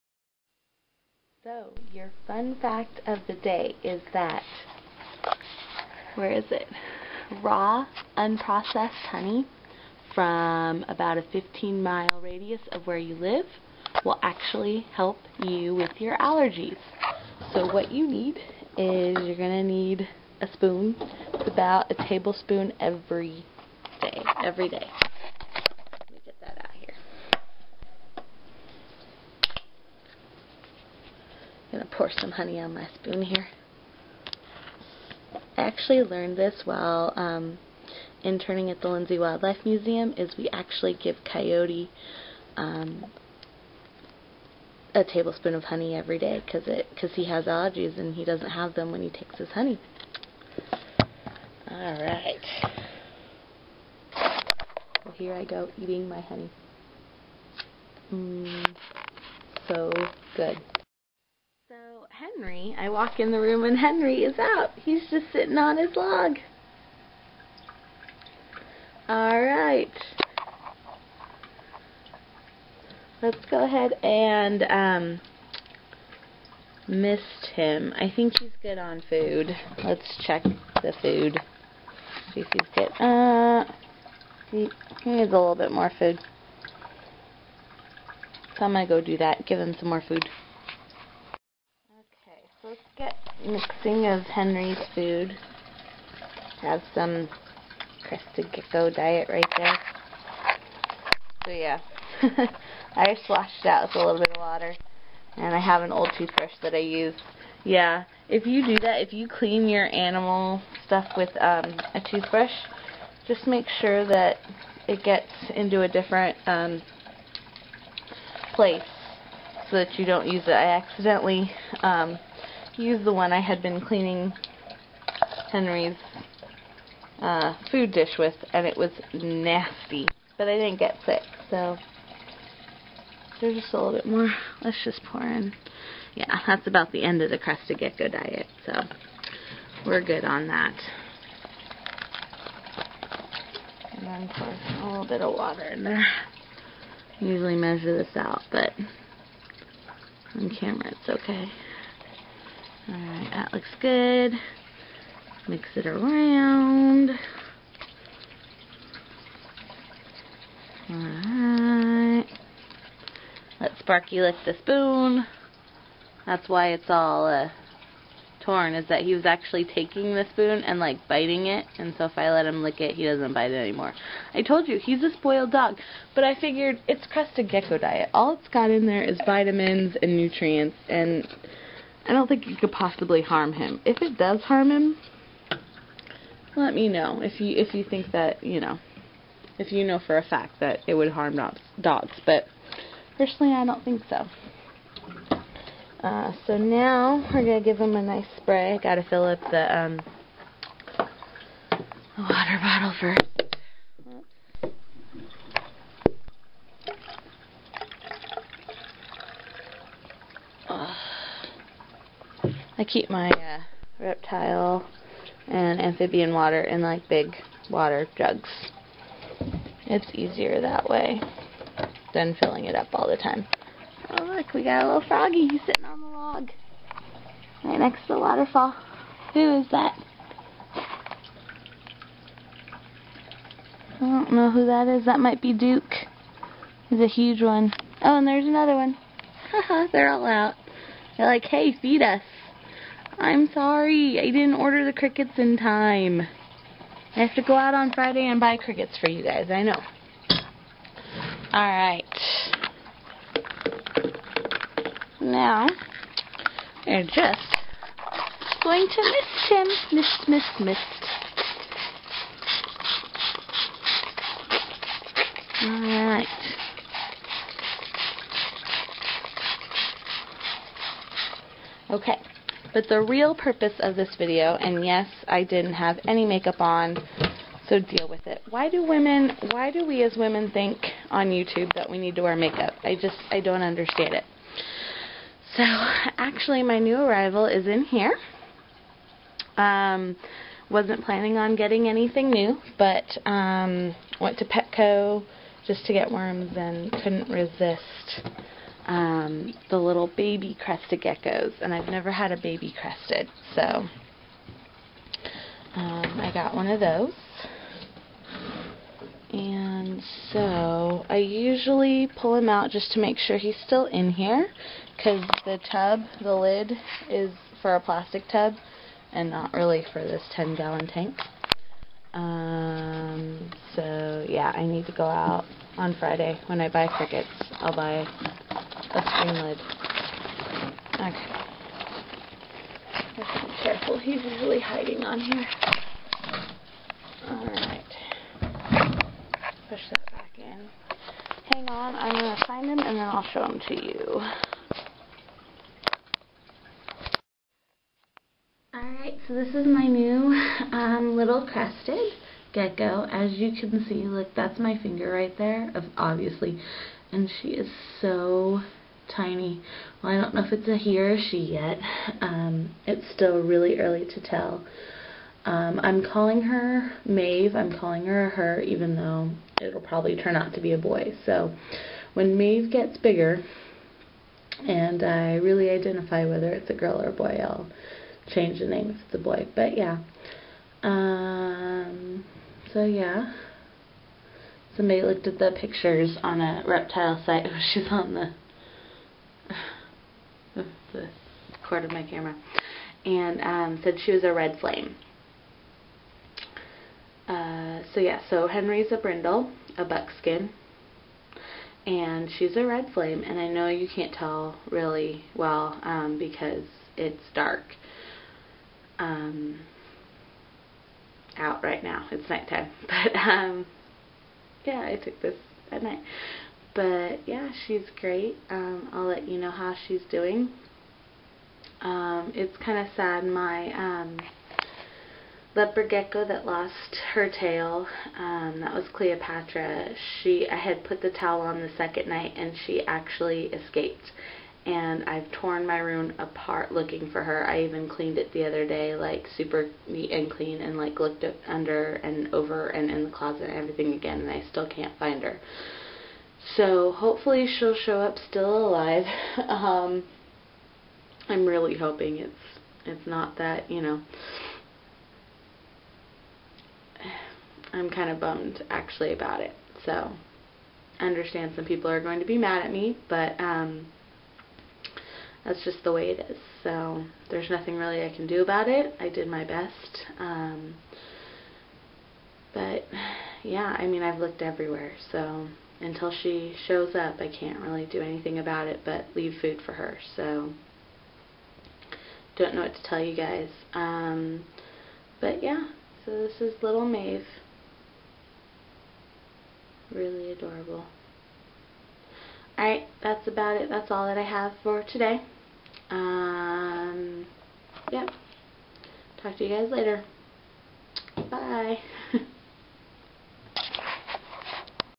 Maeve and other things.
Meet Maeve my new crestie.  This is not a Thanksgiving video,even though I uploaded it on Thanksgiving.  With that being said, Happy Thanksgiving to everyone.